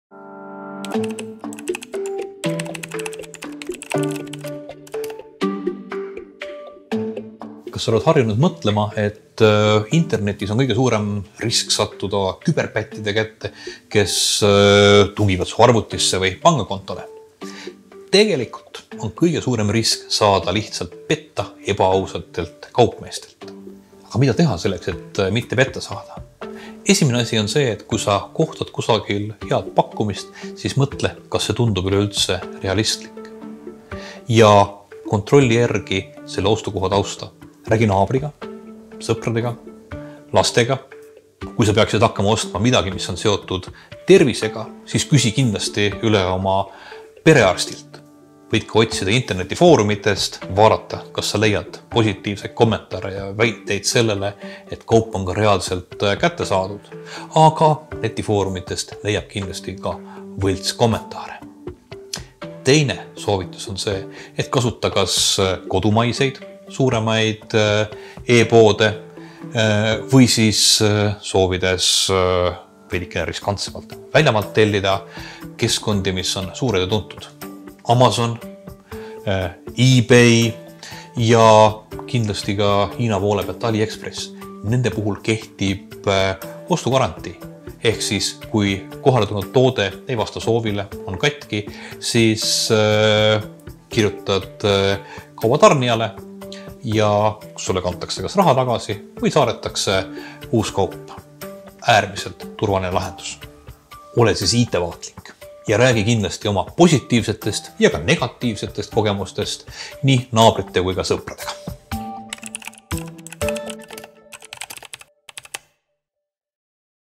Kuidas veebist ostes mitte petta saada?
Võõraste veebipoodide suhtes tuleb olla mõnevõrra skeptiline ja ettevaatlik. Eesti e-poed kannavad silti "Turvaline e-pood", kuid välismaa e-poodidel sellist märget me ei näe. Kuidas siis veenduda, et see e-pood sind ei tüssa?

Kui pakkumine on liiga hea, et olla tõsi, siis ilmselt see ongi pettus.

Uuri e-poe tausta sõpradelt, lastelt, naabrilt, netifoorumitest – kas kellelgi on sealt tellimusega kogemusi?

Suhtu skeptiliselt veebipoes olevatesse arvustustesse – neid on lihtne võltsida.

Kasuta tuntud poode, mille puhul on ostugarantii.

Kampaaniat rahastatakse Euroopa Liidu struktuuritoetuse toetusskeemist „Infoühiskonna teadlikkuse tõstmine“ (Euroopa Regionaalarengu Fond).